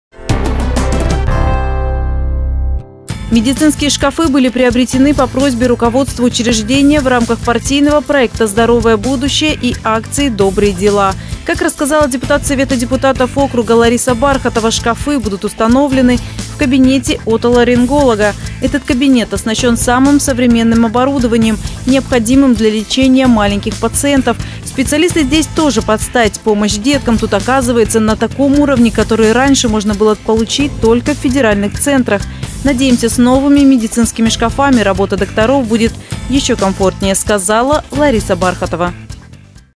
Медицинские шкафы
Одинцовские партийцы передали детской поликлинике медицинские шкафы. Они были приобретены по просьбе руководства учреждения в рамках партийного проекта «Здоровое будущее» и акции «Добрые дела». Как рассказала депутат Совета депутатов округа Лариса Бархатова, шкафы будут установлены в кабинете отоларинголога. - Этот кабинет оснащен самым современным оборудованием, необходимым для лечения маленьких пациентов.